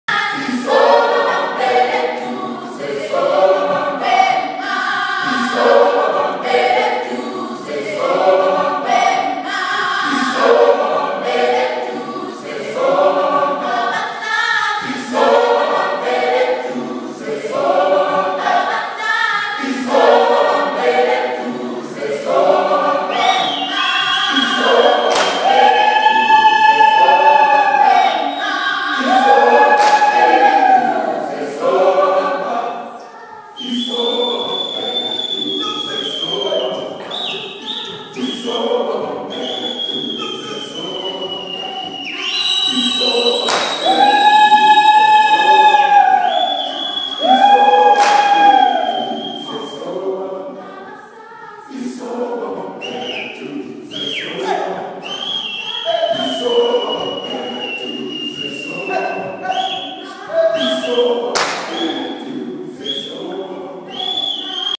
Dloko High School Choir at the Edinburgh Fringe 1
The choir performing at St Andrew's and St George's West on 6th August 2014